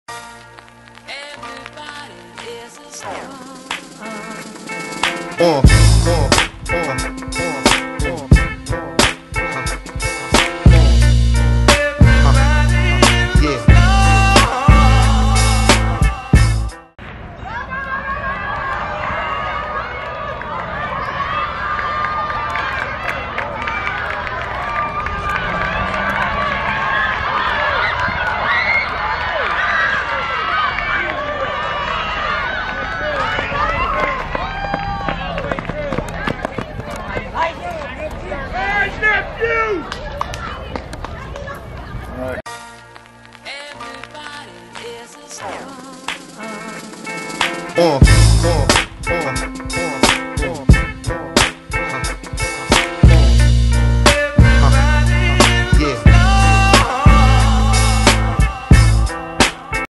APS Middle School Championship Boys 100 Hurdles
City of Akron Middle School track championship Boys 100 meter Hurdles